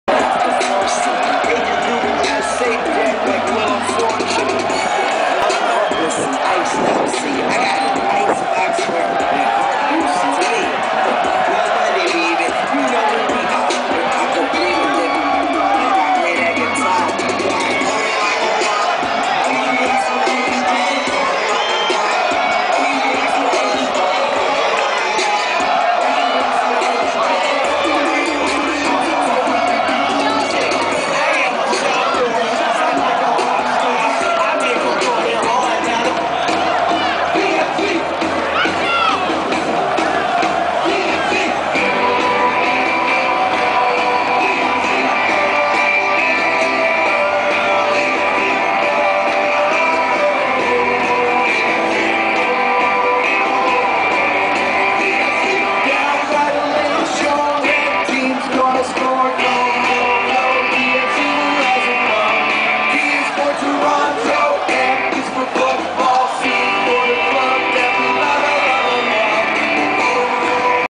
Pitch Invasion @ BMO Field
The last game of Toronto FC's inagural season ended in a tie, but you wouldn't know it from the reaction of the crowd!